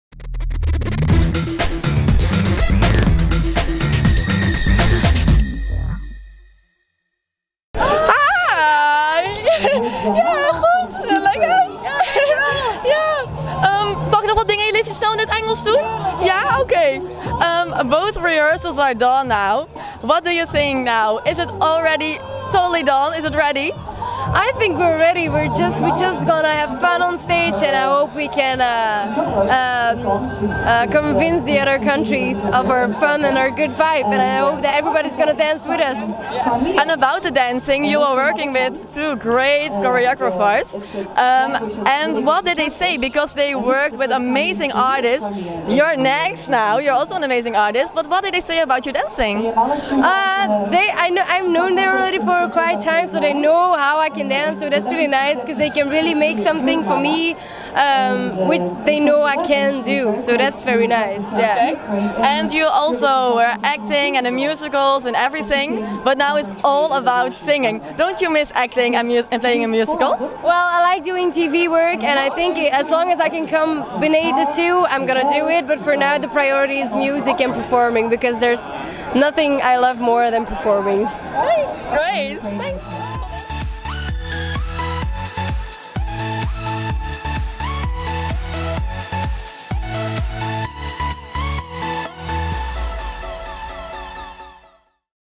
Laura Tesoro Belgium @ Eurovision 2016 red carpet | wiwibloggs
We talk to Belgium’s 2016 representative, Laura Tesoro, on the red carpet.